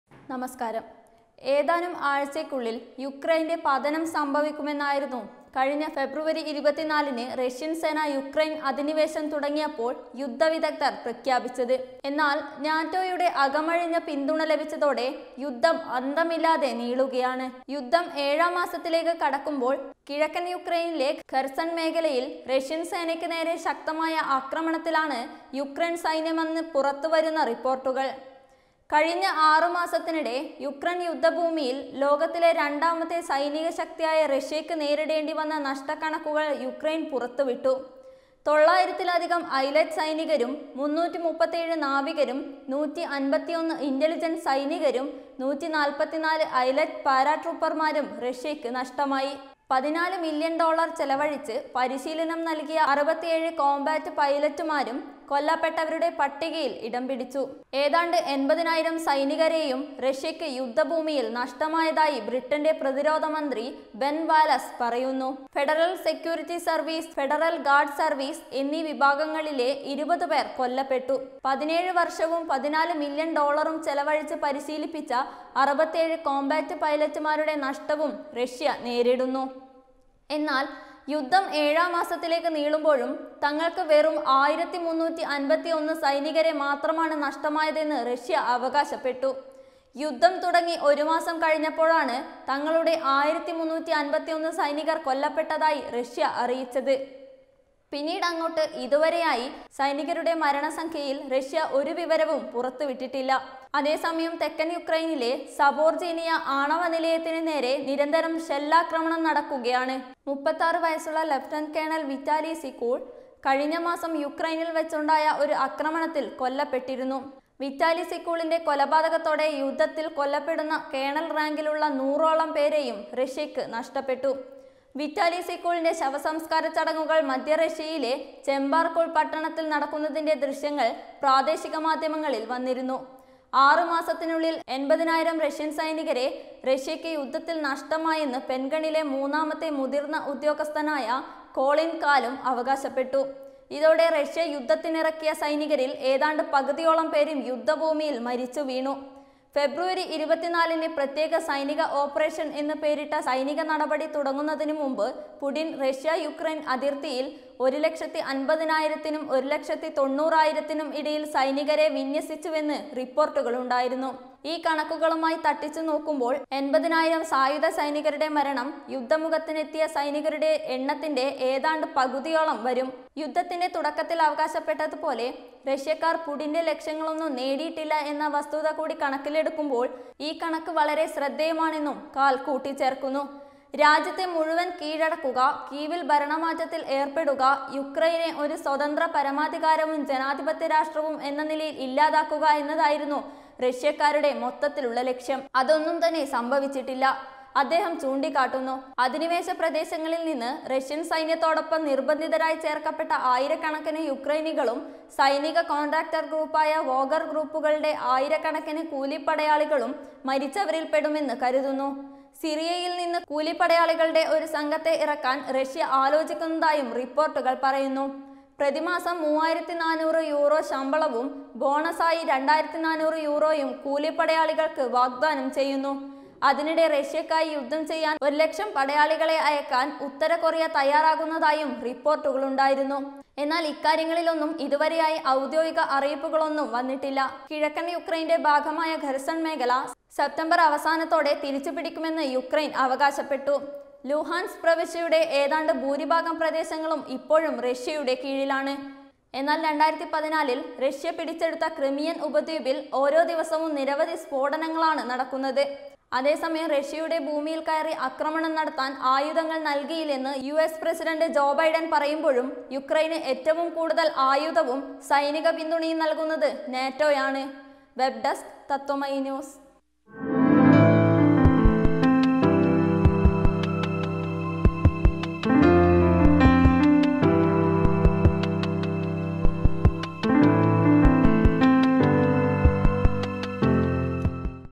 അടി തെറ്റിയോ? അതോ അമളി പറ്റിയോ? ലോകത്തെ പിടിച്ചു കുലിക്കിയ സംഘർഷം ഇനിയെങ്ങനെ? | RUSSIA
ഏതാനും ആഴ്ചകള്‍ക്കുള്ളില്‍ യുക്രൈന്‍റെ പതനം സംഭവിക്കുമെന്നായിരുന്നു കഴിഞ്ഞ ഫെബ്രുവരി 24 ന് റഷ്യന്‍ സേന യുക്രൈന്‍ അധിനിവേശം തുടങ്ങിയപ്പോള്‍ യുദ്ധവിദഗ്ദര്‍ പ്രവചിച്ചത്.